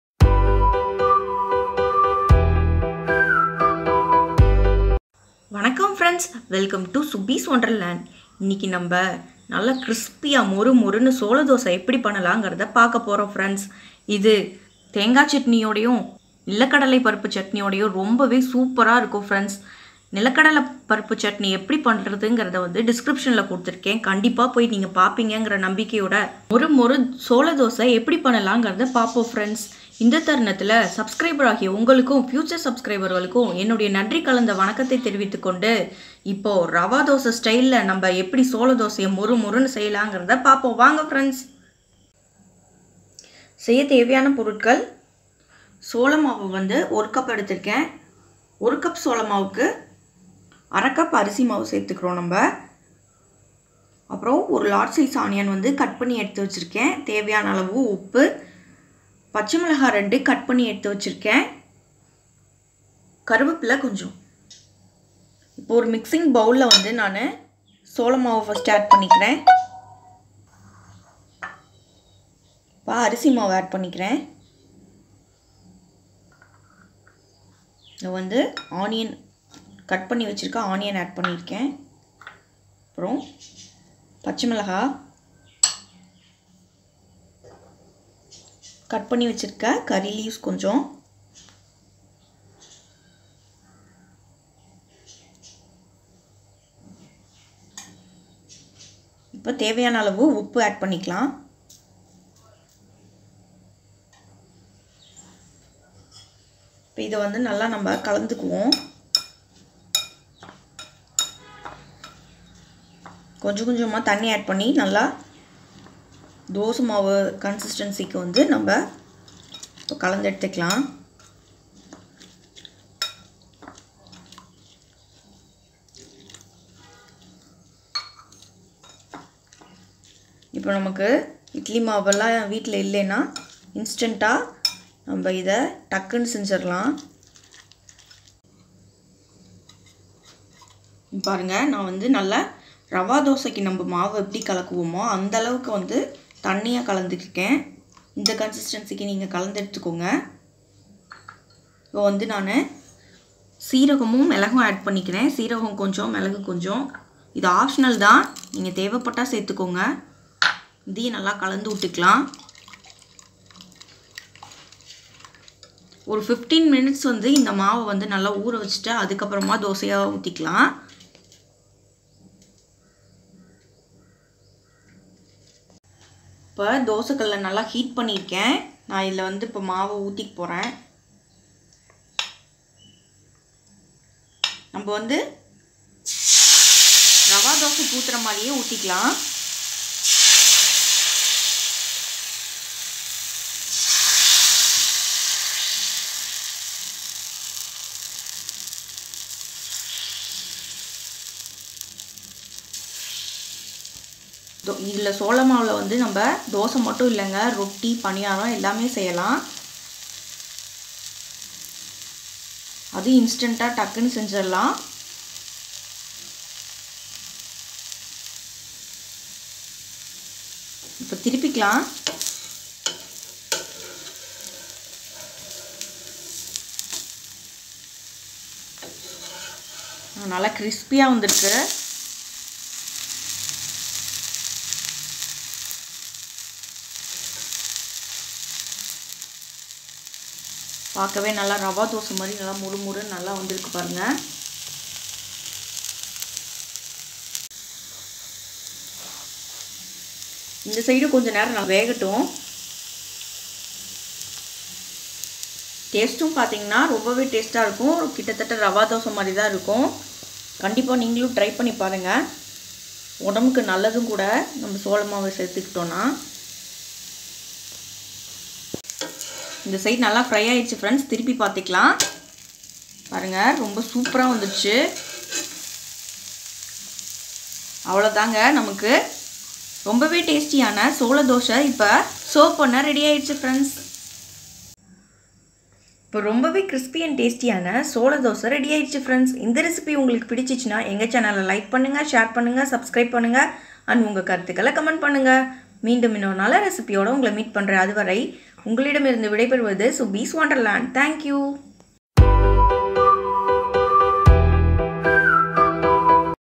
சோள தோசை/Crispy Chola Dosai Recipe/Jowar Dosa Recipe
Chola dosai is a healthy and tasty breakfast or dinner recipe which can be prepared instantly. And its taste similar as rava dosai.

chola adai recipe in tamil,chutney for chola dosai, sola maavu in tamil

Ingredients
Jowar flour - 1 cup
Rice flour - 1/2 cup
Large size onion - 1